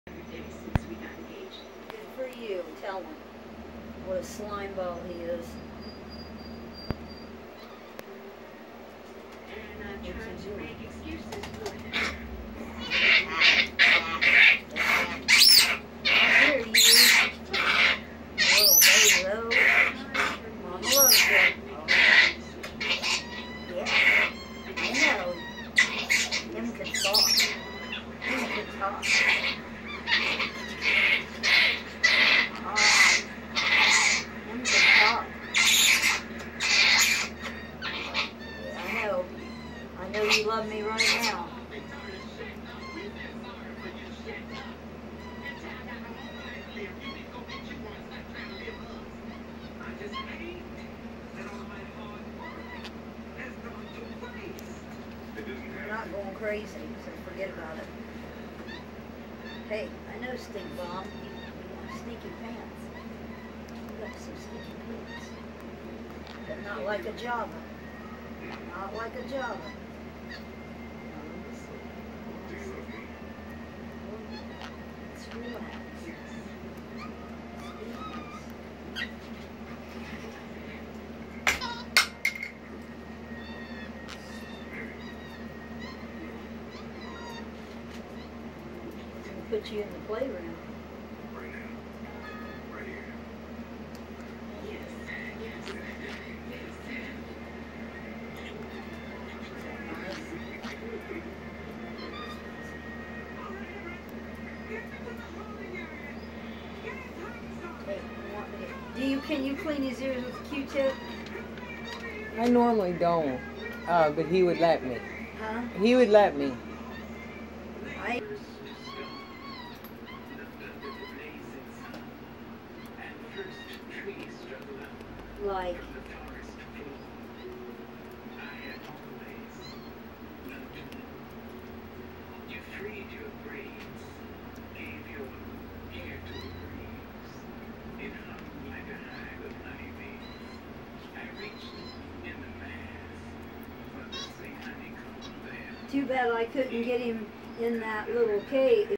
Monkey Grooming by Hannah May
Java Macaque Hannah May Day is all about the lip smacking LOVE.. She dishes out her special style of grooming while blowing the sweetest kisses the whole time.. She sends some hard core serious ASMR tingles through my body with each tap.. I love her..Always a pleasure visiting.. Life is too short to fit in!!